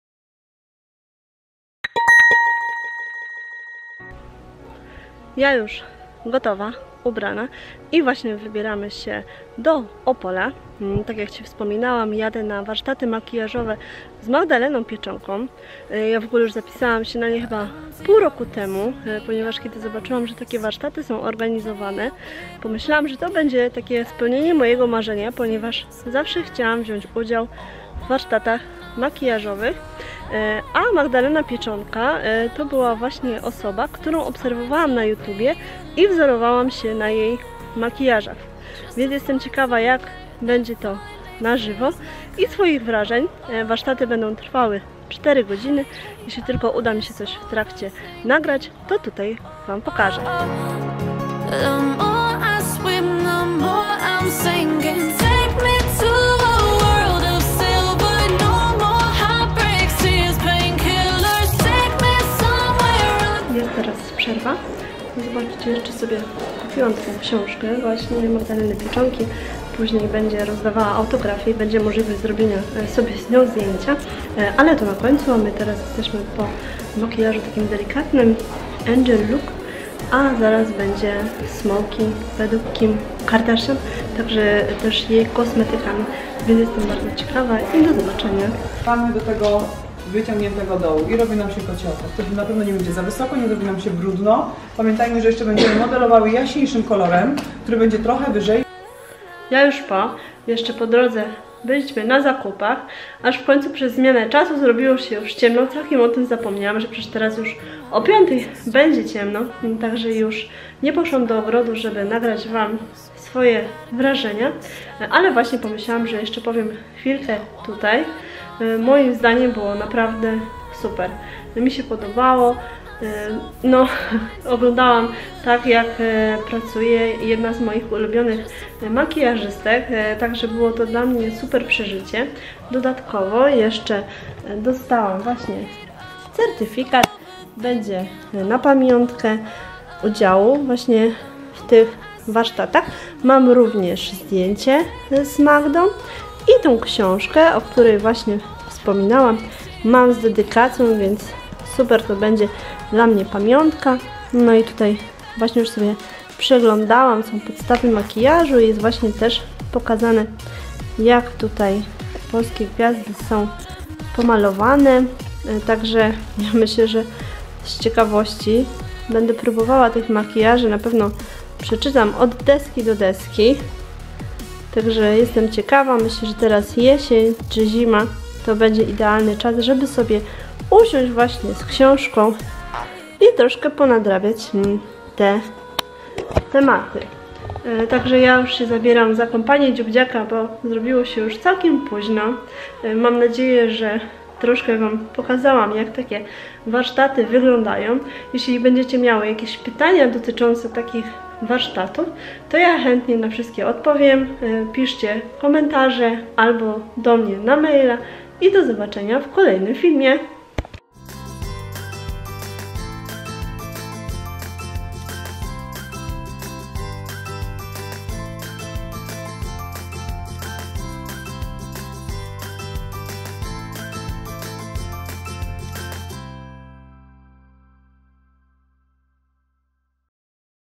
Warsztaty z Magdaleną Pieczonką - czas dla siebie :)
W październiku spełniło się moje marzenie i wzięłam udział w warsztatach makijażu :) Nie jest to zupełnie moja branża - chciałam zrobić coś dla siebie :) Jednocześnie filmy Magdaleny Pieczonki śledziłam od dawna na YouTube, a akurat warsztaty odbywały się niedaleko, bo w Opolu :)

Dlatego zachowałam wspomnienia na pamiątkę i też by pokazać Ci co nieco kulisów takich warsztatów. Już zdradzę, że polecam :) I na pewno wybiorę się ponownie, a szczegóły w filmie :)